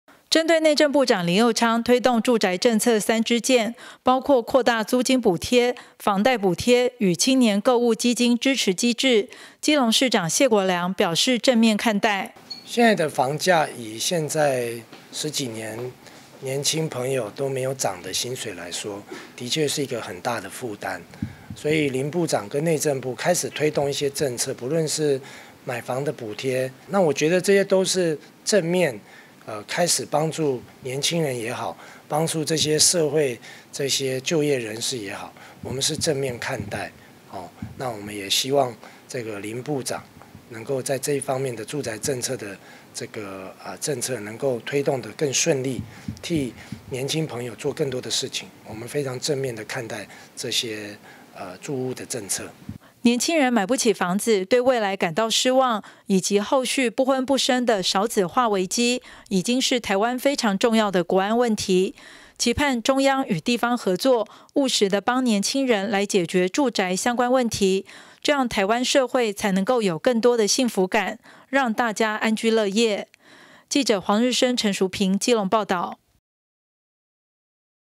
謝國樑|基隆市長|林右昌|內政部長|林右昌推住宅政策 謝國樑正面看待
針對內政部長林右昌推動住宅政策三支箭，包括擴大租金補貼、房貸補貼與青年購屋積金支持機制，基隆市長謝國樑表示正面看待。謝國樑表示，薪水10幾年沒漲，房價對年輕人來說的確是很大的負擔，內政部及林右昌部長開始推動住宅政策，開始幫助年輕人與就業人士，他正面看待，也希望林部長政策推動順利，替年輕朋友做更多事情。

而年輕人買不起房子，對未來感到失望，以及後續不婚不生的少子化危機，已經是台灣非常重要的 國安問題，期盼中央與地方合作，務實的幫助年輕人來解決住宅相關問題，這樣台灣社會能才能有更多幸福感，讓大家安居樂業。